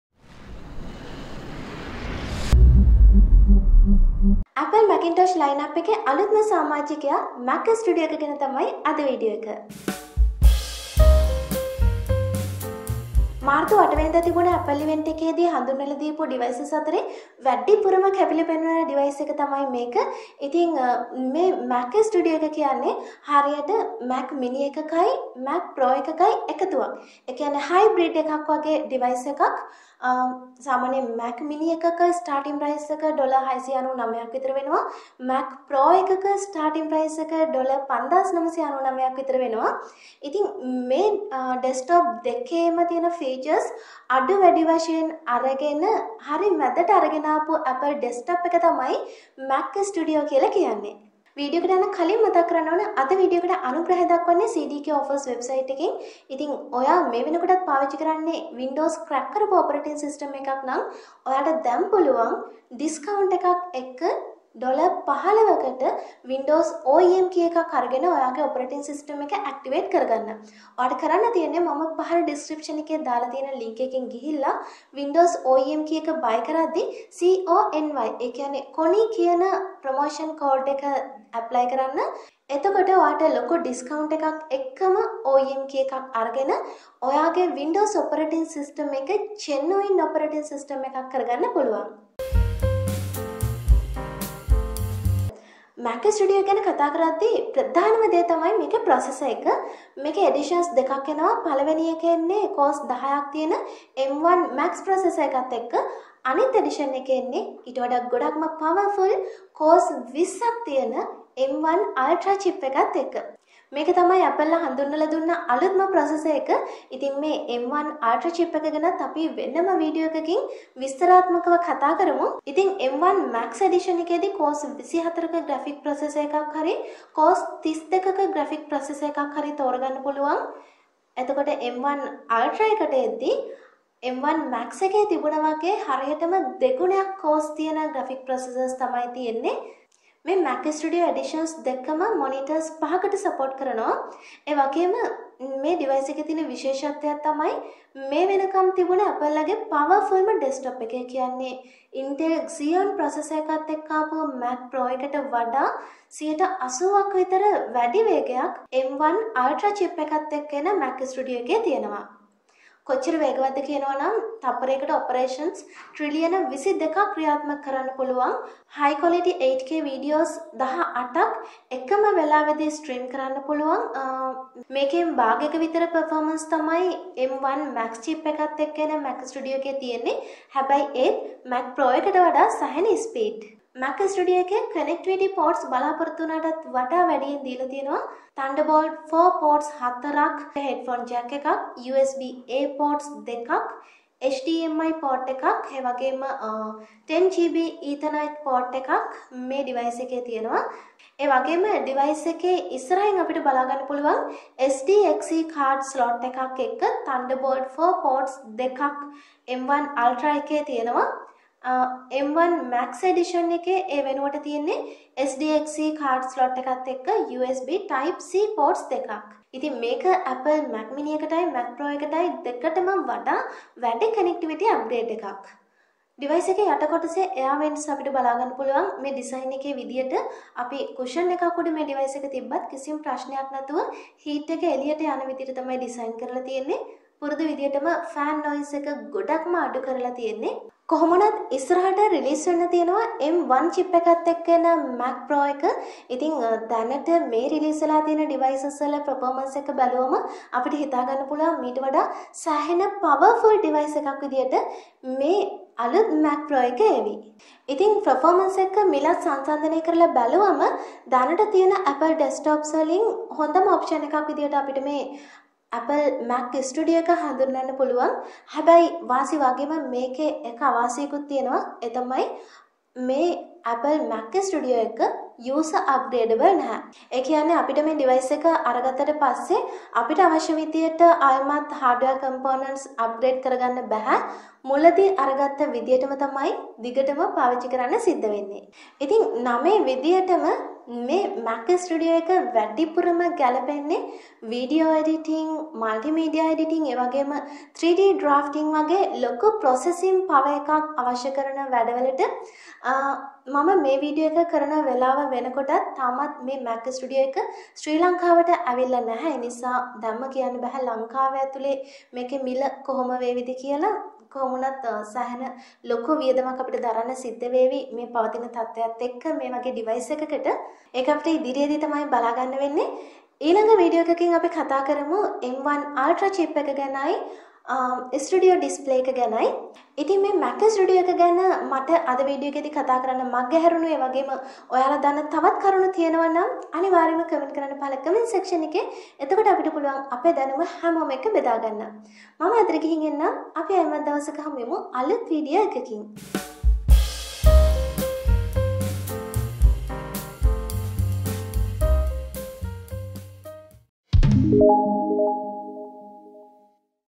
Apple MAC Studio overview
Mac Studio is an entirely new Mac desktop. It packs outrageous performance, extensive connectivity, and new capabilities into an unbelievably compact form, putting everything you need within easy reach and transforming any space into a studio. And it all starts with your choice of the ferociously fast M1 Max or the all-new M1 Ultra — the most powerful chip ever in a personal computer.

ඇපල්  මැකින්ටොෂ් ලයිනප් එකේ අලුත්ම සාමාජිකයා, මැක් ස්ටුඩියෝ එක ගැන තමයි අද වීඩියෝ එක. මාර්තු අට වෙනිදා ඇපල් ඉවෙන්ට් එකේදී හදුන්වල දුන්න අලුත් ඩිවයිසස් අතරේ වැඩිපුරම කපිල පෙනුන ඩිවයිස් එක තමයි මේ. මේක හරියටම කිව්වොත් mac mini එකෙයි මැක් ප්‍රෝ එකෙයී එකතුවක්, හයිබ්‍රිඩ් එකක් වගේ. මැක් මිනි එකක ස්ටාර්ටින් ප්‍රයිස් එක $699 ක් විතර වෙනවා. මැක් ප්‍රෝ එකක ස්ටාර්ටින් ප්‍රයිස්   $5,999 ක් විතර වෙනවා. මේ දෙකෙම ෆීචර්ස් අඩු වැඩි විදියට අරගෙන අතරමැදට ගෙනාපු ඩෙස්ක්ටොප් එක තමයි මැක් ස්ටුඩියෝ කියන්නේ.

Mac Studio ගැන කතා කරද්දී ප්‍රධානම දේ තමයි Processor  එක. මේකේ editions දෙකක් එනවා. එකක් එන්නේ cores දහයක් තියෙන M1 Max ප්‍රොසෙසර් එකත් එක්ක. අනිත් එඩිෂන් එක මීට වඩා පවර්ෆුල් කොර්ස් විස්සක් එක්ක එන M1 Ultra ප්‍රොසෙසර් එක. මේ ප්‍රොසෙසර් එකත් හදුනවලා දුන්නේ මාර්තු අට වෙනිදමයි.

එතකොට M 1 මැක්ස් එකේ cores 24 graphic processor එකක් හරි 32 ක Graphic processor එකක් හරි  තෝරාගන්න පුළුවන්.  M1 Ultra එකේ හරියටම එකේ දෙගුණයක් cores තියෙන  Graphic processor  එකක් තියෙනවා.

මේ Mac Studio editions දෙකම ඩෙස්ක්ටොප් එක මොනිටර්ස් පහකට සපෝර්ට් කරනවා. ඉතින් මෙතන විශේෂත්වය තමයි මේ වෙනකම්ම තිබුන අපල්ලගේ පවර්ෆුල්ම ඩෙස්ක්ටොප් එක ඒ කියන්නේ  Intel Xeon ප්‍රොසෙසර් එකත් එක්ක ආව Mac Pro එකට වඩා 80%ක විතර වැඩි වේගයක් ඊට වඩා අඩු මිලට එන m1 ultra චිප් එකත් එක්ක එන මැක් ස්ටුඩියෝ එකේ තියෙනවා.
කොච්චර වේගවත්ද කියනවා නම් තත්පරයකට Operations ට්‍රිලියන 22ක් ක්‍රියාත්මක කරන්න පුළුවන්. high-quality 8k videos  18ක් එකම වේලාවකදී  stream කරන්න මේ Mac Studio එකට පුළුවන්. මේකෙන් බාගෙක performance තමයි M1 Max Chip එකත් එක්ක එන Mac Studio එකේ තියෙන්නේ. හැබැයි එත් Mac pro එකට වඩා ස්පීඩ්.
හැබැයි ඉස්සරට එන්නේ තියෙනවා M1 Processor එකක් එක්ක එන Mac pro එක. ඒක ආවට පස්සේ ආයෙත් අලුත් Mac pro එක වඩාත්ම powerful වේවි. එතකම් මේක තමයි වැඩිපුර ස්පීඩ් වෙන්නේ.

Mac Studio එකේ Connectivity ports බලාපොරොත්තු උනාටත් වඩා වැඩියෙන් දීලා තියෙනවා. Thunderbolt 4 ports 4ක් ,3.5 මිලිමීටර් හෙඩ්ෆෝන් jack  එකක්,  USB-A ports දෙකක් , HDMI Port එකක් 10Gb Ethernet port එකක්  මේ ඩිවයිස් එකේ තියෙනවා. මෙතැනදී තවත් අමතරව SDXC card slot එකක් සහ  තන්දර්බෝල්ට් 4 පොර්ට්ස් දෙකක් එම් 1 අල්ට්‍රා එකේ ඉස්සරින් තියෙනවා. M1 Max එඩිෂන් එකේ ඒ වෙනුවට තියෙන්නේ  SDXC card slot එකත් එක්ක USB type C ports දෙකක්.
ඉතින්  මේක Apple Mac Mini සහ Mac pro දෙකටම වඩා වැඩි Connectivity upgrade එකක්.

Device එක යට කොටසේ Air vents අපිට බලාගන්න පුළුවන්. මේ Design එකේ විදියට අපි කුෂන් එකක් උඩ මේ Device  එක තිබ්බත් කසිම ප්‍රශ්නයක් නැතුව Heat එක එලියට යන විදියට ඩිසයින් කරලා තියෙන්නේ. පුරුදු විදියට fan noise එක අඩුම කරලා තමයි තියෙන්නේ.
ඉතින් පර්ෆෝමන්ස් අතින් ගත්තොත් දැනට තියෙන apple desktop ඔප්ෂන්ස් වලින් වඩාත්ම හොද ඔප්ෂන් එක විදියට අපිට මේ Mac Studio එක හදුන්වන්න පුළුවන්. හැබැයි මේ වාසි එක්කම අවාසියකුත් තියෙනවා. ඒ තමයි මේ Device එක user upgradable නැහැ. ඒ කියන්නේ අපිට මේ Device එක අරගත්තට පස්සේ Hardware upgrades අපිට ඕන විදියට කරන්න පුළුවන් කමක් නැහැ. මුලින්ම අරගත්ත විදියටම තමයි හැමදාම පාවිච්චි කරන්න වෙන්නේ.

ඉතින් නමේ විදියටම මේ Device  එක වැඩිපුරම ගැලපෙන්නේ Studio පර්පස් වලට. ඒ කියන්නේ Video Editing, Multimedia  වගේ සෑහෙන ලොකු Processing power එකක් අවශ්‍ය වැඩ වලට. කොහොම වුනත් පවතින තත්වයත් එක්ක මේ Device එක කවදා විතර ලංකාවේ වෙළදපලට නිකුත් වේවිද කියන්නත් අමාරුයි. එක අපිට ඉස්සරහට තමයි බලන්න වෙන්නේ. ඉතින් ඉස්සරහට එන වීඩියෝස් වලින් අපි කතා කරමු M1 Ultra chip එක සහ Studio Display එක ගැනත්. ඉතින් මේ Mac Studio එක ගැන මට කතා කරන්න මගහැරුණු ඔයාලා දන්නා තවත් කරුණු තියෙනවා නම් අනිවාර්යෙන්ම කමෙන්ට් කරන්න පහල කමෙන්ට් සෙක්ෂන් එකේ. එතකොට අපිට පුළුවන් අපේ දැනුම හැමෝම එක්ක බෙදා ගන්න.